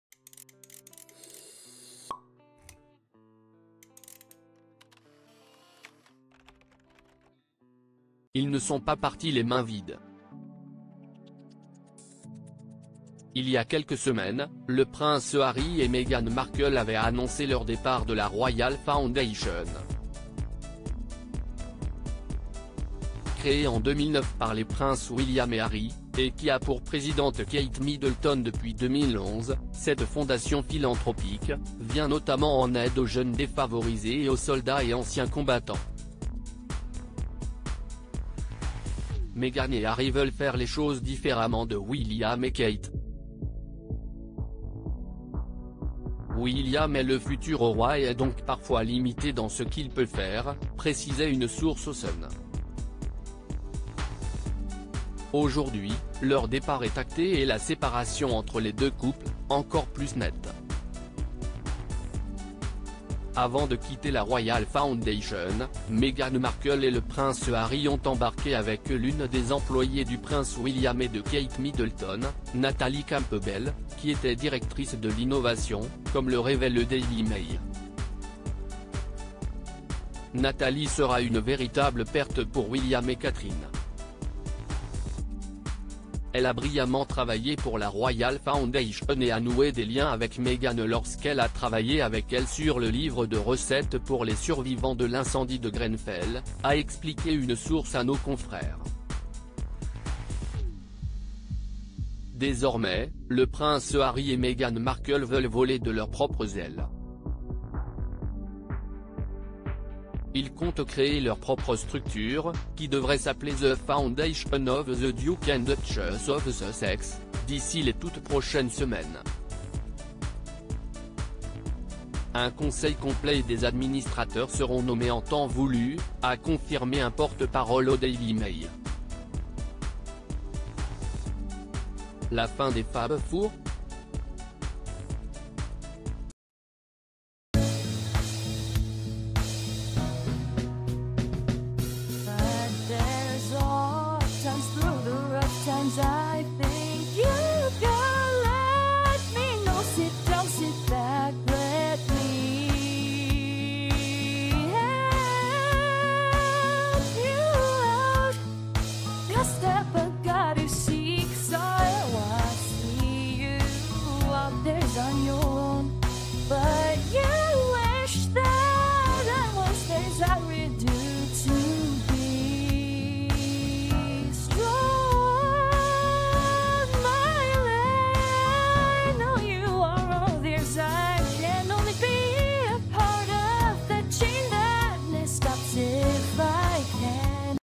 Ça ne s'arrange pas... Le prince Harry et Meghan Markle ont piqué un membre du personnel de Kate Mid
Ça ne s'arrange pas... Le prince Harry et Meghan Markle ont piqué un membre du personnel de Kate Middleton et du prince William